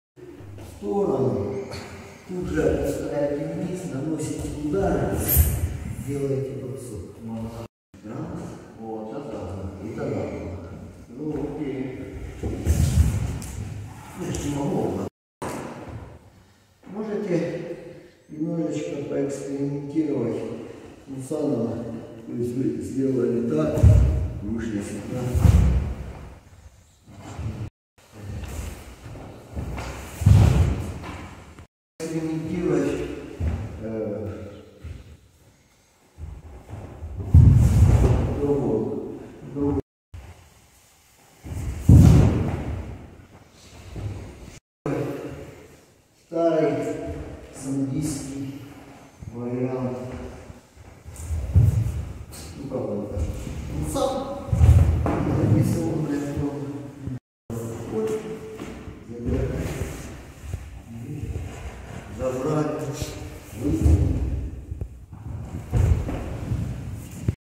Как базовые техники одной школы прекрасно накладываются на боевые другой
тренируясь с ребятами я пытаюсь продемонстрировать, как можно объединять движения. здесь начало техники стандартное, а вот окончания из других школ!!!

When training with the guys, I try to demonstrate how to combine movements. Here the beginning of the technique is standard, but the graduation from other schools!!!